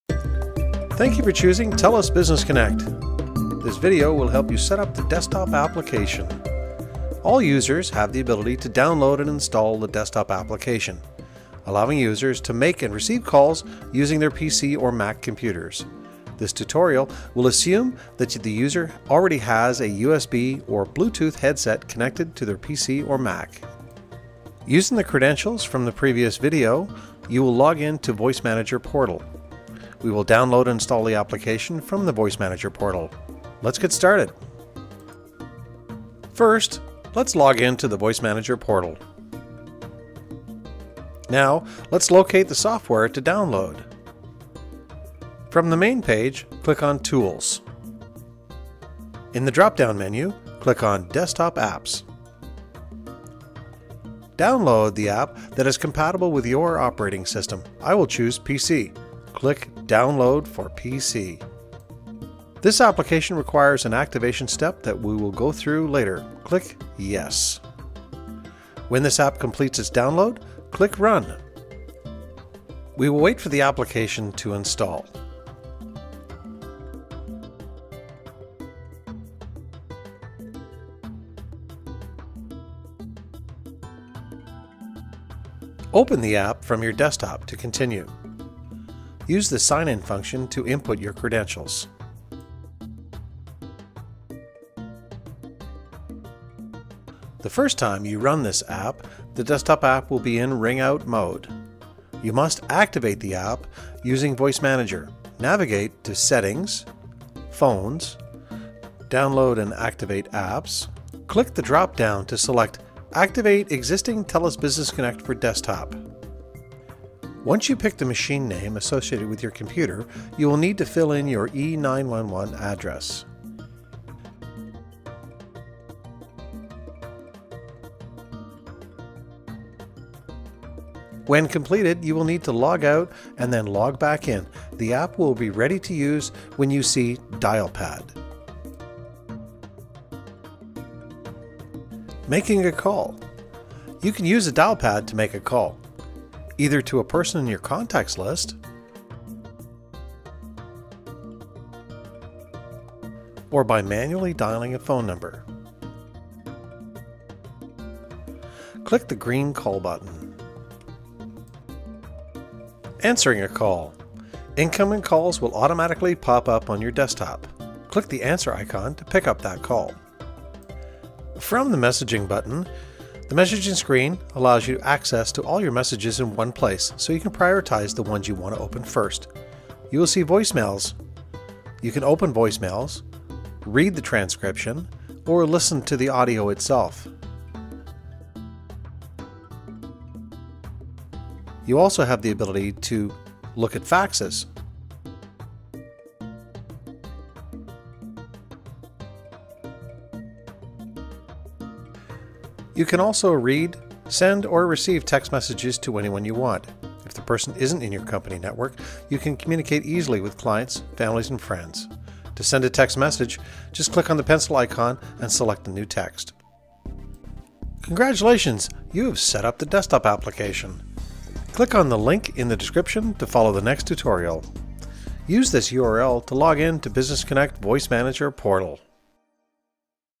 "On-boarding" - Business Connect - Desktop Application
Business Connect Desktop Application, download installation and first use.